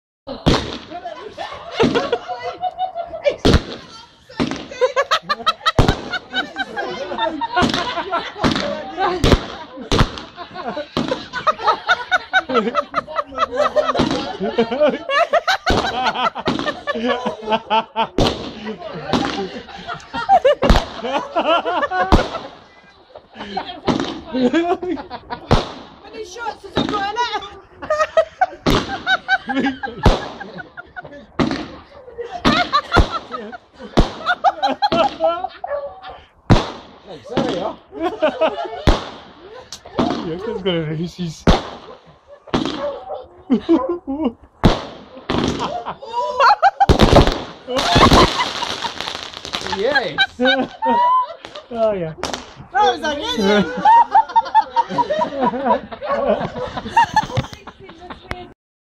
Fireworks - keep upright!!
..my mums next door neighbour had a 32 shot firework which he accidently put on it's side instead of the way is should have been. It was soooo funny, thank goodness my mums house backs onto a field! You might want to turn your speakers down - as we're all laughing and screaming like banshee's lol